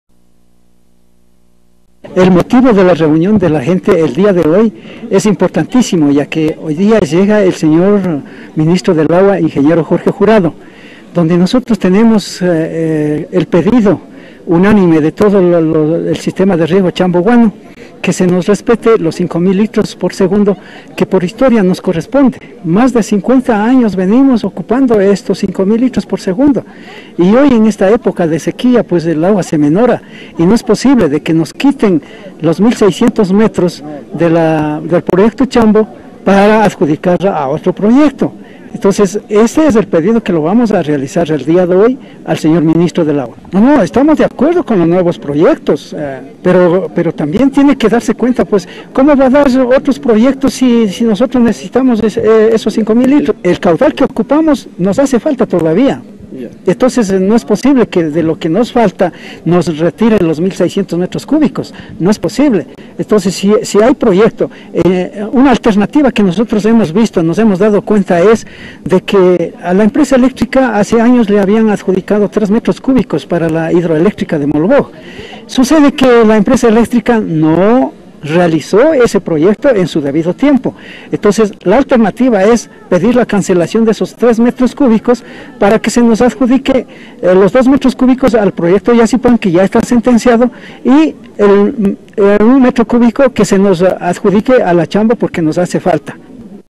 Noticias Riobamba 26/05/2009 - Beneficiarios del sistema de riego Chambo-Riobamba solicitan que no se limite caudal
Canal TVS - Los beneficiarios del sistema de riego Chambo-Riobamba, solicitaron al secretario de agua, que no se limite el caudal para el regadio.